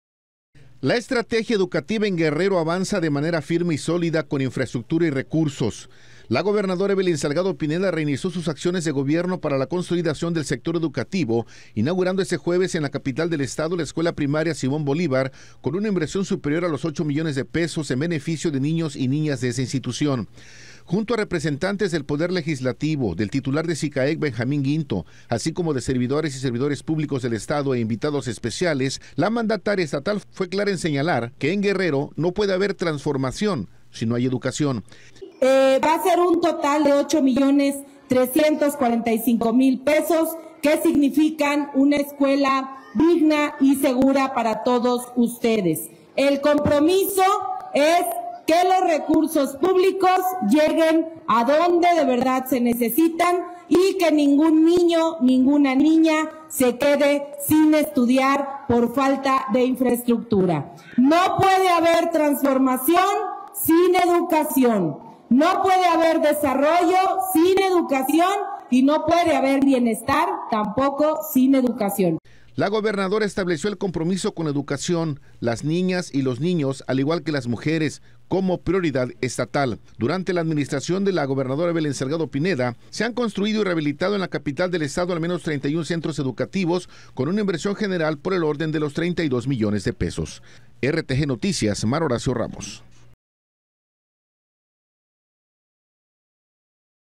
RTGnoticias | Reinicia Gobernadora acciones por la educación en Guerrero
La estrategia educativa en Guerrero avanza de manera firme y sólida, con infraestructura y recursos. La gobernadora Evelyn Salgado Pineda reinició sus acciones de gobierno para la consolidación del sector educativo inaugurando este jueves en la capital del estado, la escuela primaria Simón Bolívar, con una inversión superior a los 8 millones de pesos, en beneficio de niños y niñas desea institución.